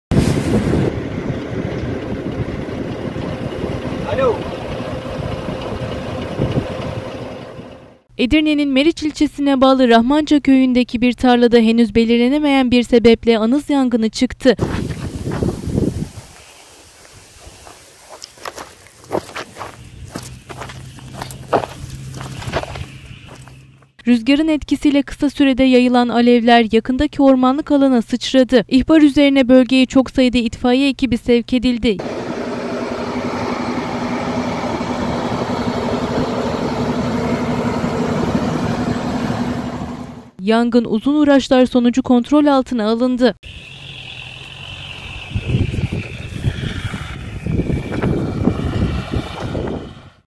ANIZ YANGINI ORMANLIK ALANA SIÇRADI
Edirne’nin Meriç ilçesinde bir tarlada çıkan ve ormanlık alana sıçrayan yangın, itfaiye ekipleri tarafından söndürüldü.